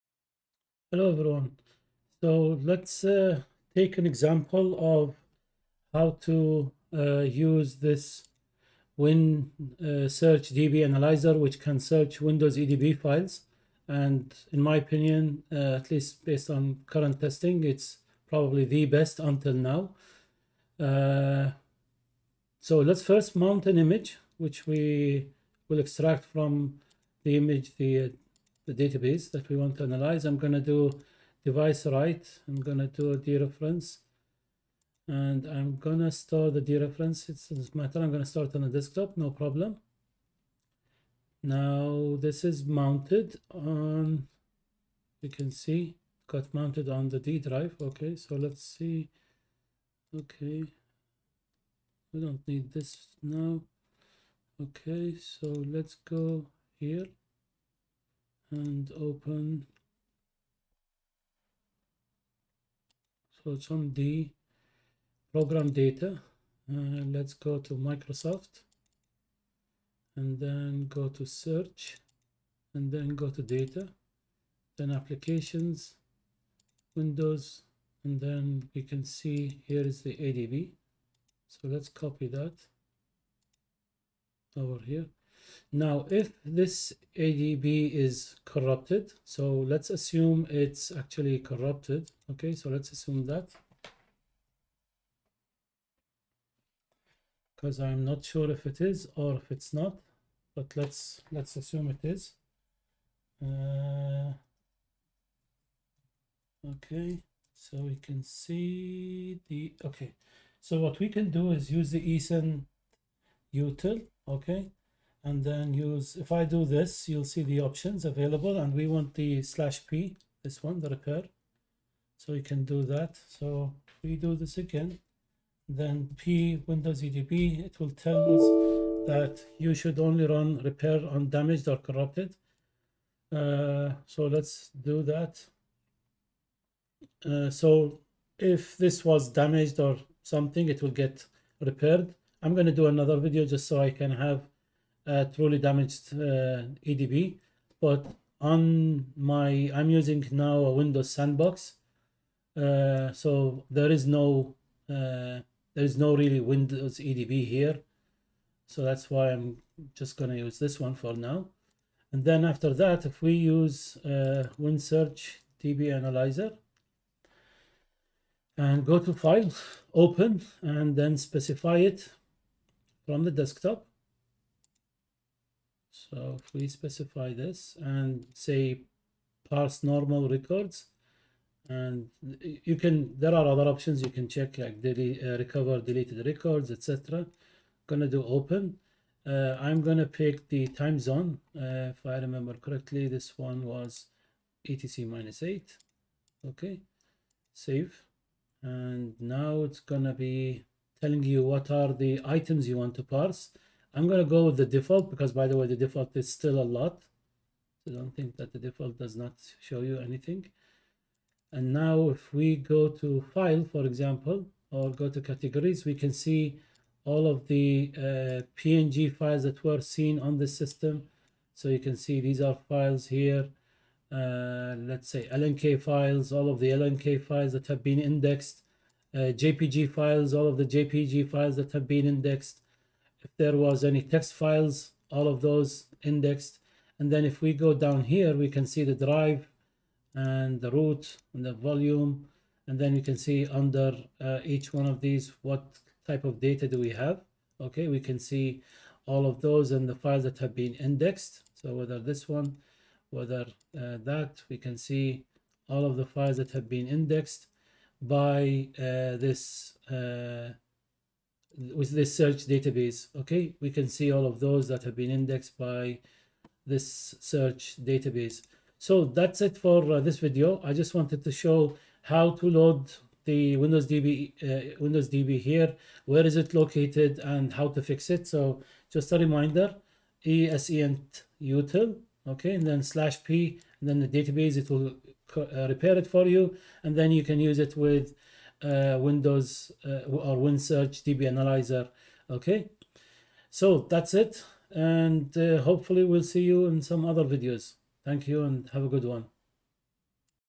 Fixing Windows edb and using WinSearchDBAnalyzer
This video shows how to copy the Windows.edb Search DB, fix it using esentutl and then load it for analysis using WinSearchDBAnalyzer.